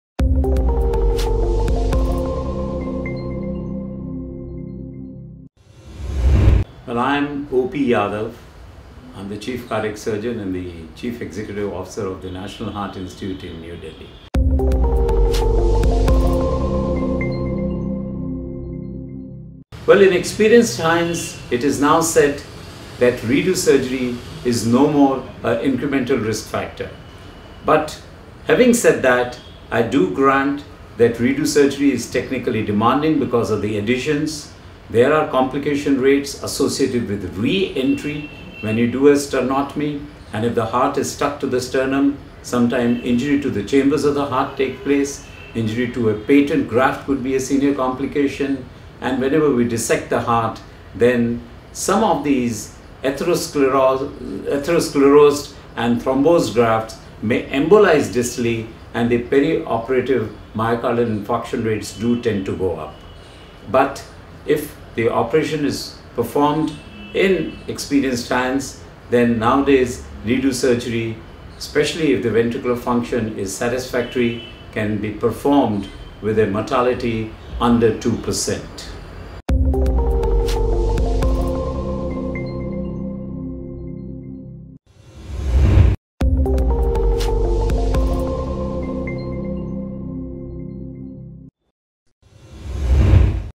How difficult is redo bypass surgery? | Dr O P Yadava | Medtalks
In this video, Dr. O P. Yadava, Chief Cardiac Surgeon, National Heart Institute talks about the advantages and complications of redo bypass surgery. Currently redo bypass surgery is technically demanding.